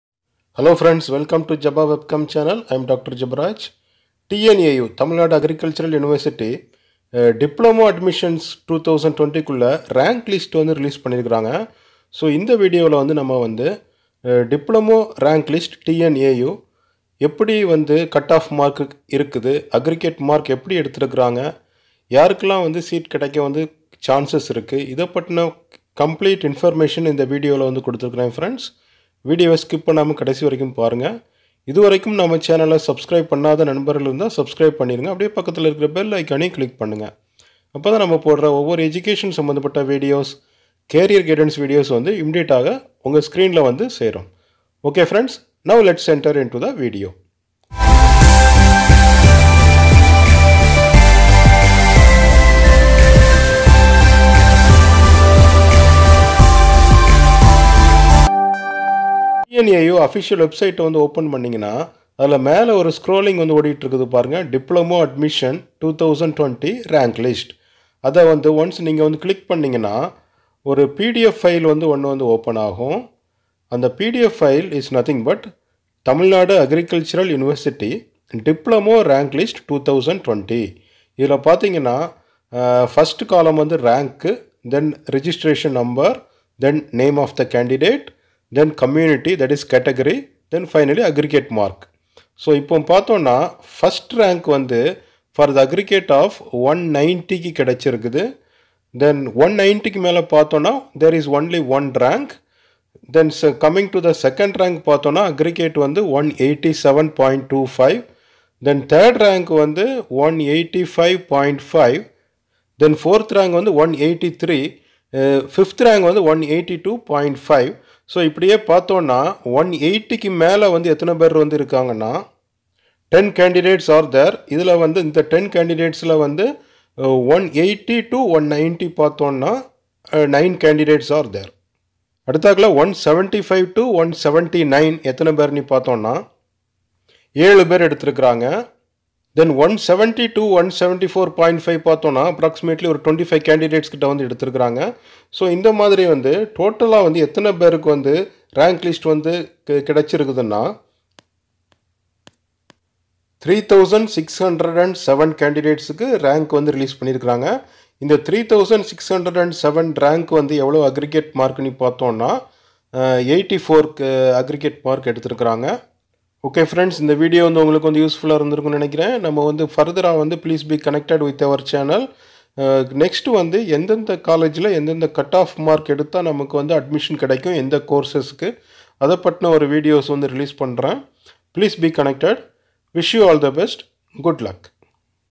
TNAU Diploma Admissions - Latest Notification | Rank List Analysis | Full Details in Tamil
In this video,  the TNAU diploma rank list 2020 is analysed.

Jeba Webcam Youtube channel has been created by Dr.Jebaraj S., M.E., Ph.D. with a vision of publishing videos in career guidance & education.